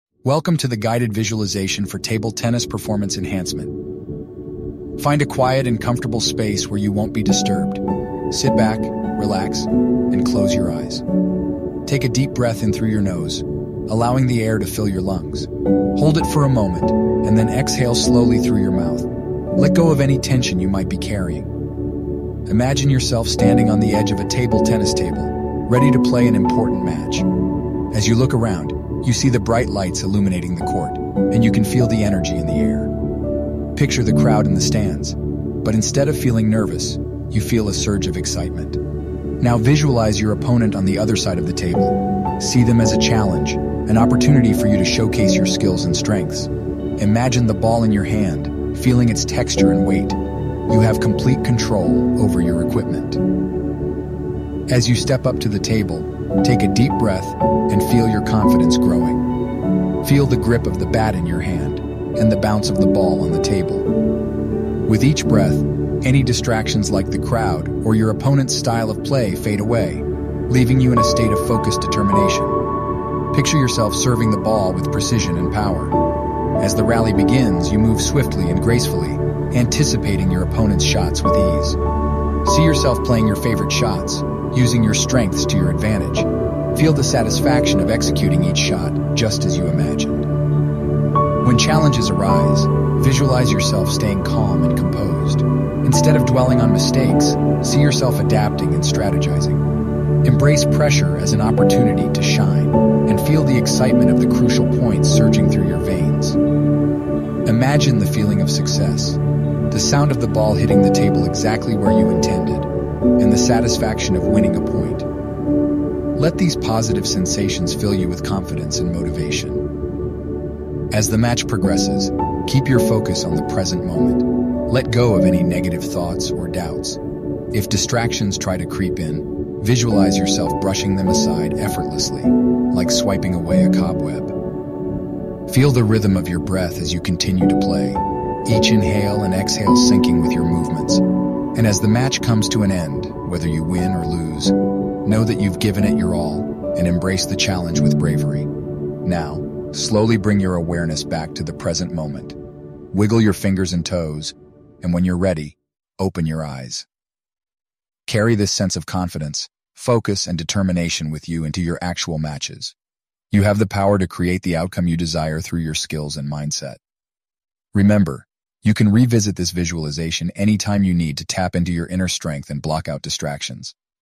Winning Starts In Your Mind - Next-Level Guided Visualisation for Table Tennis Greatness 👊🏓
Get in the zone and prepare mentally for your next big table tennis match with this 3-minute guided meditation. Listen along as we walk you through key visualisation and focus techniques used by top athletes to get centred, grounded, and dialled-in pre-match.

This simple yet powerful guided meditation will get you feeling centred, confident, and primed for table tennis greatness.

Use it before matches to get into your mental performance zone quickly and effectively.